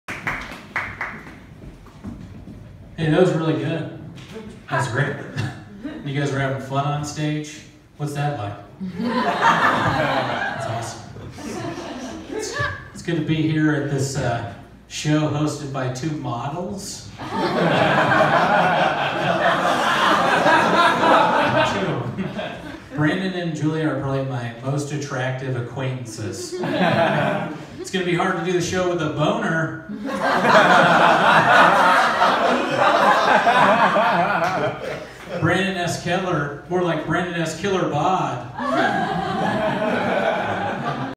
Shocking MeToo Moment Caught on Stage
Comedian goes off rails at Pointless Comedy show hosted by Julia Austin and Brandon Scott Keller. Sundays at the Clubhouse in Los Feliz.